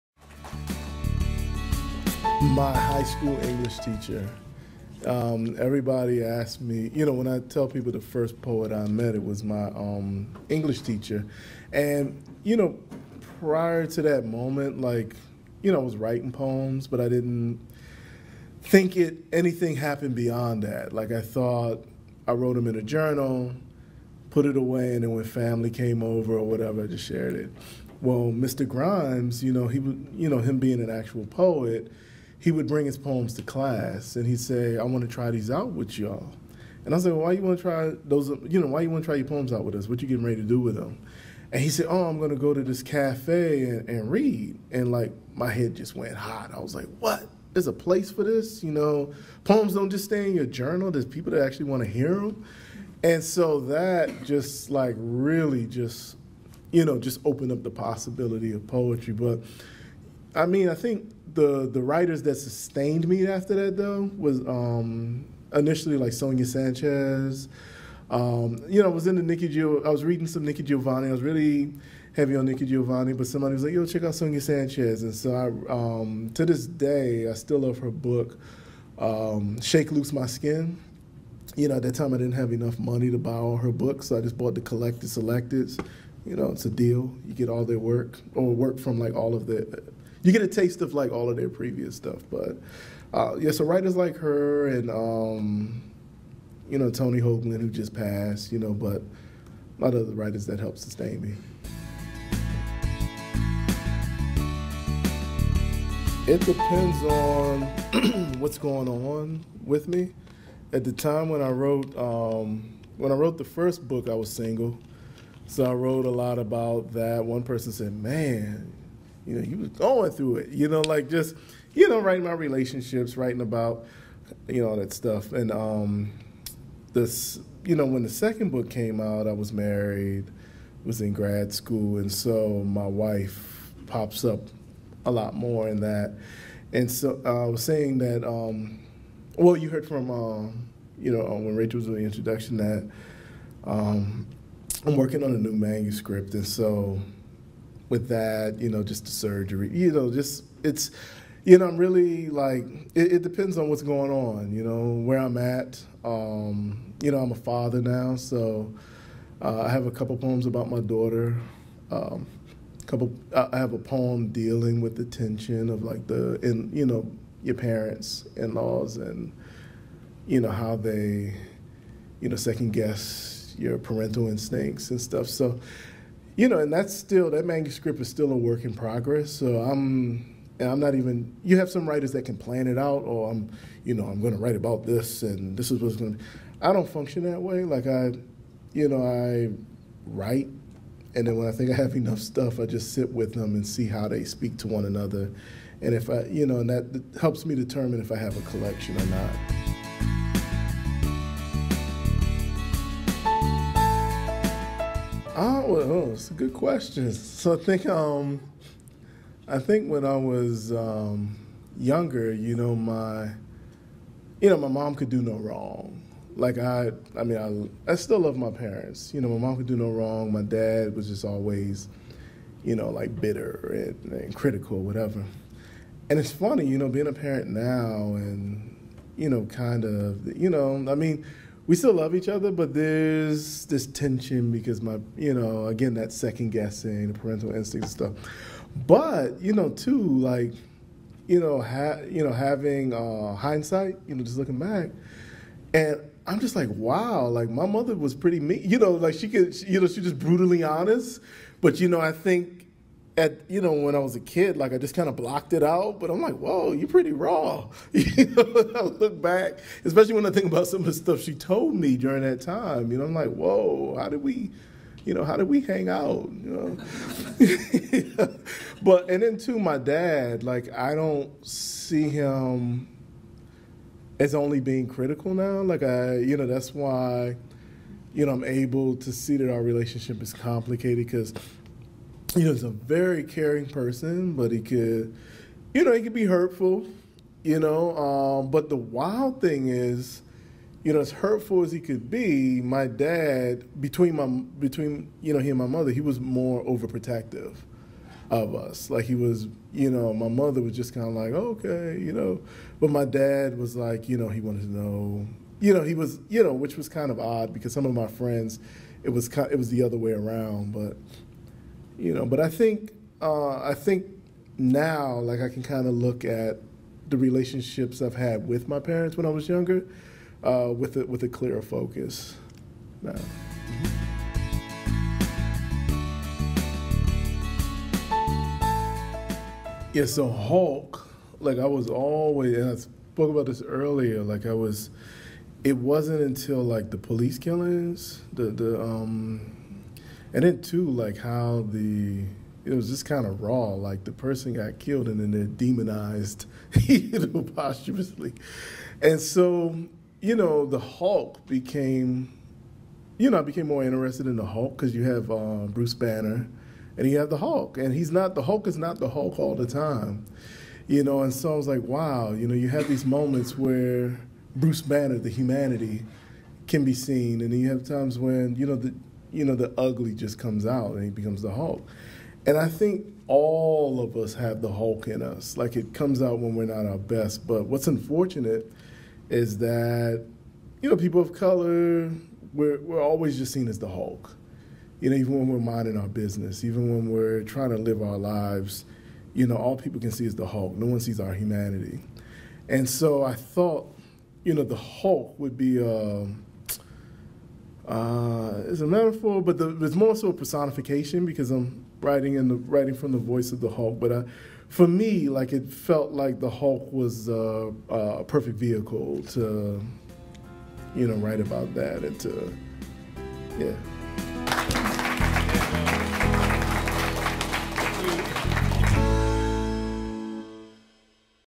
Remaining Q&A
During the Q&A, I got to talk about my high school English teacher, the first poet I ever met.